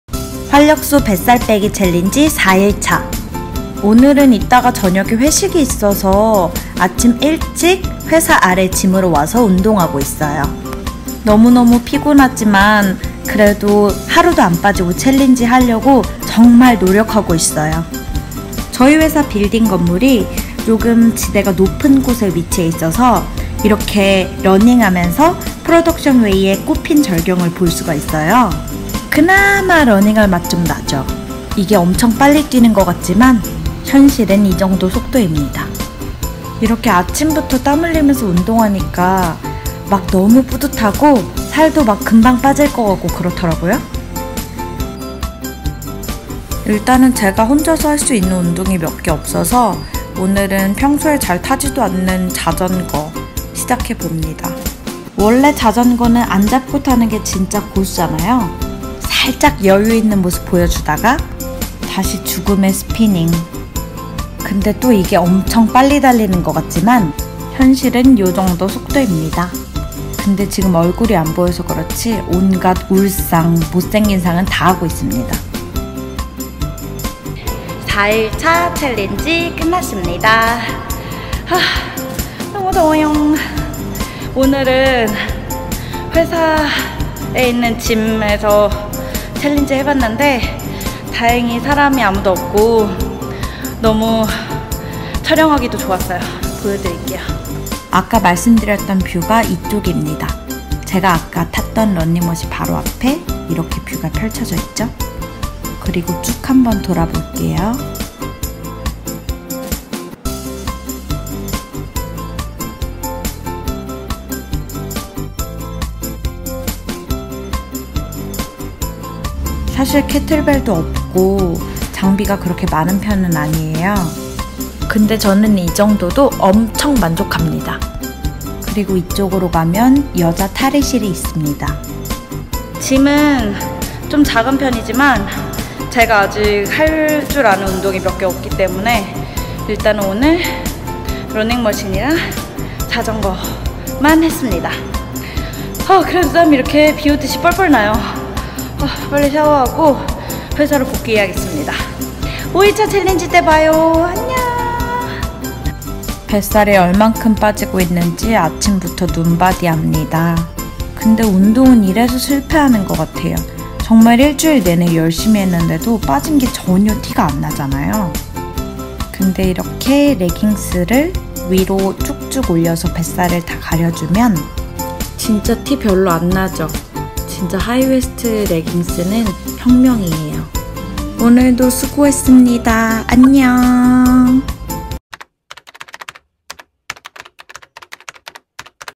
활력쏘 뱃살빼기 챌린지 4일차 / 밴쿠버 회사 헬스장 소개 / 러닝머신 20분, 스피닝 10분
챌린지 4일차!! 저녁에 회식있어서 이번엔 회사 일찍 출근해서 아침에 운동했어요!!
밴쿠버 회사 헬스장이라고 하기엔 민망할정도로 조금 작고 단촐하지만,
뷰도 좋고 탈의실도 잘되어있고 자주 올거같아요 :)

운알못인 저는 오늘 러닝머신과 스피닝만으로 챌린지 마쳤습니다!
그럼 우리 5일차때 봐용!